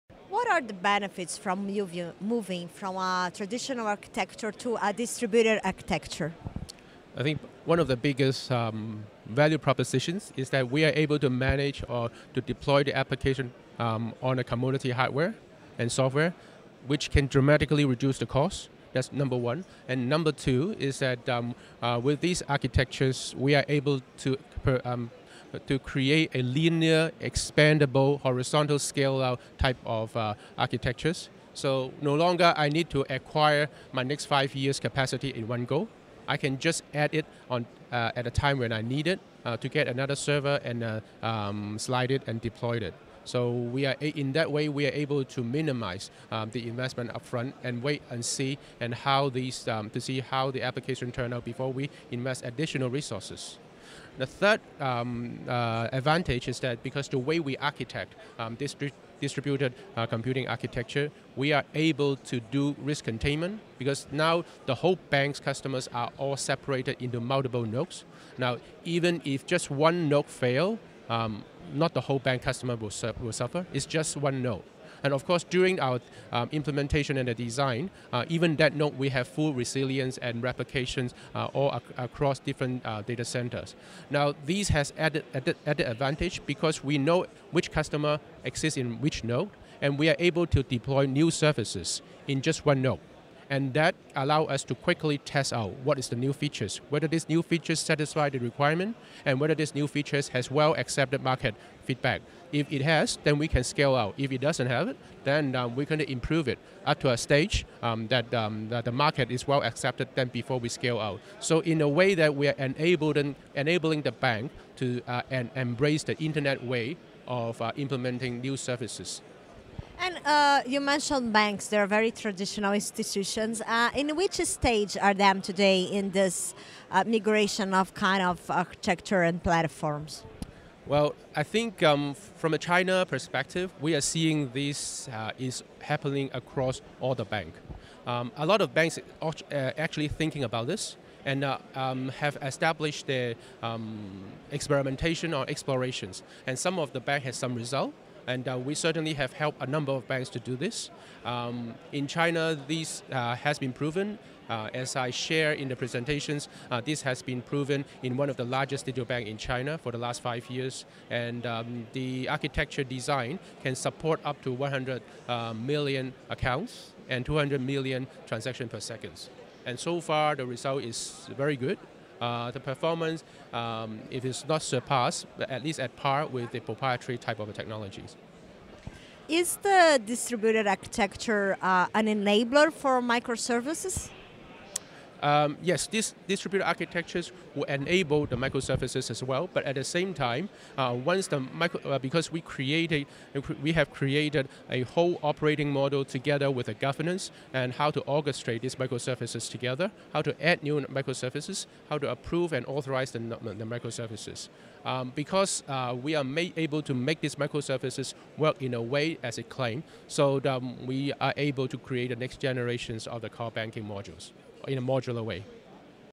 Arquitetura distribuída: uma tendência que vai chegar ao Brasil
As empresas encontram benefícios ao migrar de uma arquitetura tradicional para uma distribuída, conforme explicou Daniel Leung, chefe de fintechs e arquitetura na Forms Syntron, em entrevista à CDTV, durante o Ciab Febraban 2019, realizado de 11 a 13 de junho, em São Paulo. Segundo ele, uma das principais proposições de valor da arquitetura distribuída é implementar e gerenciar as aplicações nas camadas de software e hardware, reduzindo custo, e também adicionar capacidade conforme a necessitar.